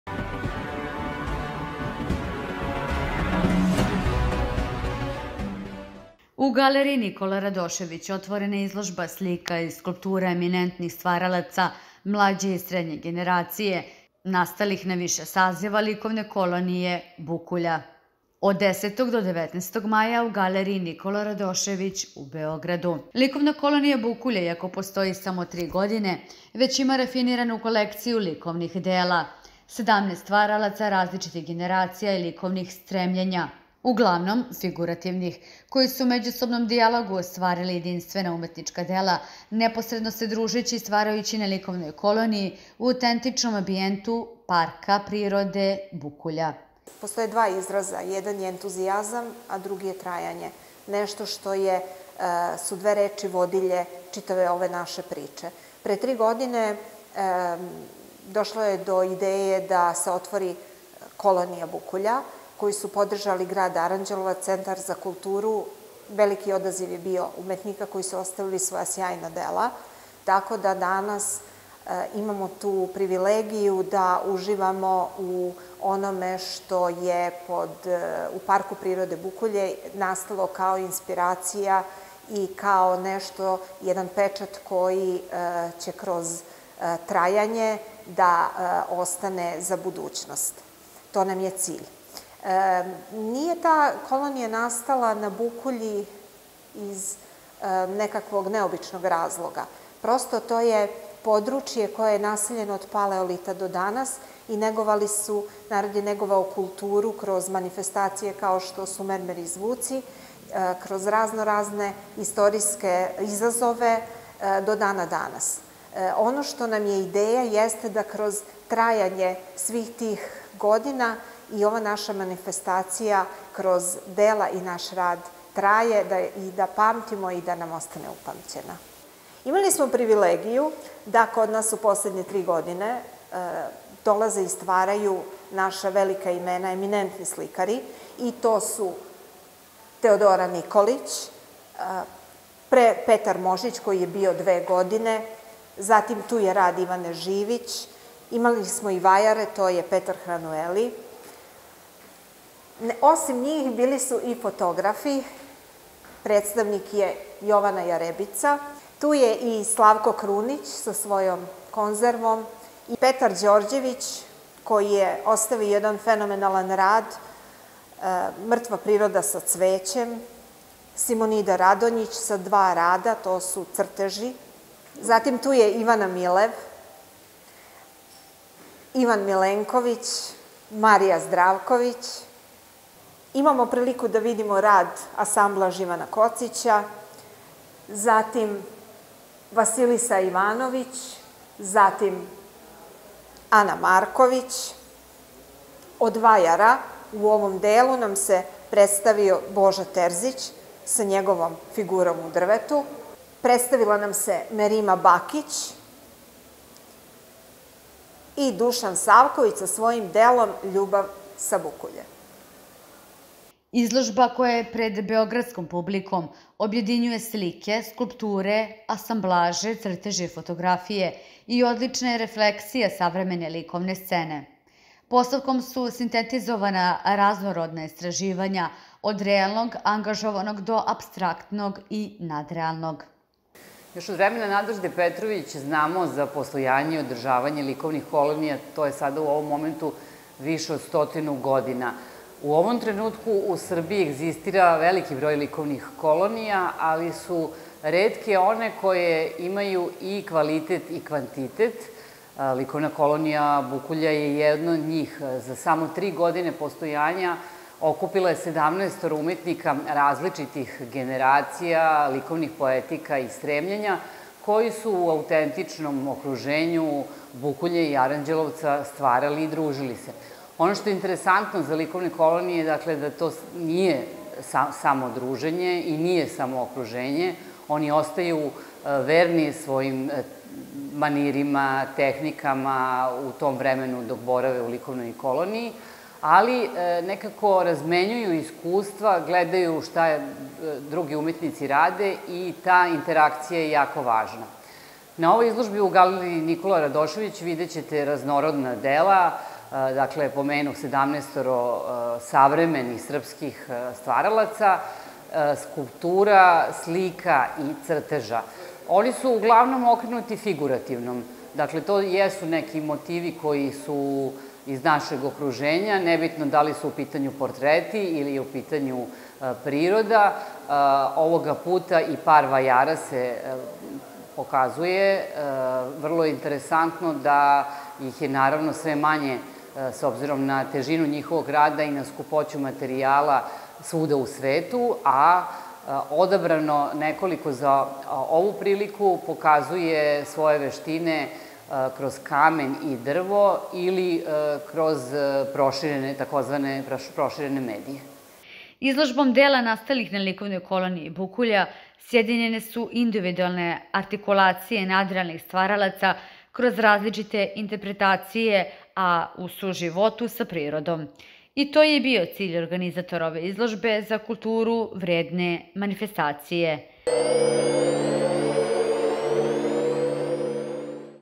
LIKOVNA KOLONIJA BUKULJA - u galeriji "Nikola Radošević"
LIKOVNA KOLONIJA   BUKULJA

     U galeriji "Nikola Radošević" otvarana je  izložbe slika i skulptura eminentih stvaralaca mlađe i srednje generacije, nastalih na više saziva likovne  kolonije "Bukulja", od: 10. do: 19. maja - u galeriji "Nikola Radošević" u Beogradu.
      Likovna kolonija "Bukulja", iako postoji samo tri godine, već ima rafiniranu kolekciju likovnih dela: 17 stvaralaca, različitih generacija i likovnih stremljenja (uglavnom figurativnih) koji su u međusobnom dijalogu ostvarili jedinstvena umetnička dela, neposredno se družeći i stvarajući na likovnoj koloniji - u autentičnom ambijentu parka prirode Bukulja.
Izložba koja je pred beogradskom publikom objedinjuje slike, skulpture, asamblaže, crteže i fotografije i odlična je refleksija savremene likovne scene. Postavkom su sintetizovana i raznorodna izražavanja...od realnog, angažovanog do apstraktnog i nadrealnog.
     Slikarstvo raznolikih tehnika i poetika je najzastupljenije, a ima tematiku širokog dijapazona - od portreta i pejzaža, preko floralnog do apstraktnog.
Preovlađuju portreti, od realističnih do nadrealnih. Aktuelne i bolne teme odrastanja u monohromatskim prizorima okosnica su radova Petra Mošića, Ivan Kocić je prepoznatljiv po tehnici asamblaža jarkog kolorita, nastalih od recikliranih igračaka, Slavko Krunić smešta devojke renesansne lepote u konzerve, a Merima Bakić impresionističkim pristupom dočarava sunčan letnji dan.  Dušan Savković u delu svedene, simbolističke forme je bio inspirisan lepotama Bukulje, a Ivan Milenković svetlošću tehnologije sažima modernu komunikaciju na detalj.
Rafiniran i kontinuiran pristup floralnom, nežnog kolorita i mikro - makro odnosa ima Marija Zdravković, a Petar Đorđević daje savremeno viđenje mrtve prirode u vizuelno atraktivnim slikama.
Crtež kao najintimniju likovnu tehniku, kroz asocijativne forme, ali i prikaze čudesnih bića neguju Simonida Radonjić i Vasilisa Ivanović.
Teodora Nikolić u fragmentima ljudske kože vidi suštinu, a Ivana Živić rafiniranim pristupom izmešta autentične plivačice iz realnog u vanpojavno.
Apstraktne intencije smirenog kolorita karakteristične su za Anu Marković, koja je i jedina predstavnica takvog načina likovog izražavanja na ovoj postavci.
     Skulpturama manjih formata, u kamenu i drvetu, sa animalnim i figuralnim motivima - predstavili su se Petar Hranueli i Božo Terzić, a Ivana Milev postornom instalacijom.
Jovana Jarebica kroz medij fotografije istražuje telesnost i svetlost, nagoveštaj i pokret.
Izložbom dela nastalih na likovnoj koloniji "Bukulja" - sjedinjene su individualne artikulacije nadarenih stvaralaca kroz različite interpretacije, u suživotu sa prirodom... i to je i bio cilj organizatora ove izložbe za kulturu vredne manifestacije.